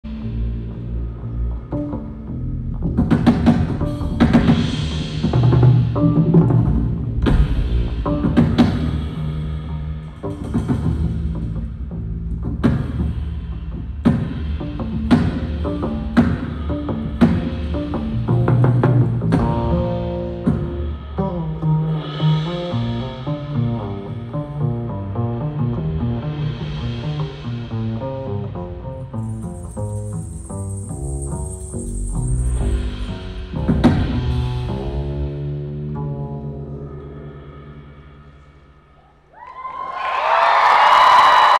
Laufey's Band - Jazz Break | Goddess Tour LIVE @ The Masonic 4.12.24
Laufey LIVE @ The Masonic, April 12 @ 8:00pm
Goddess Tour feat. Grace Enger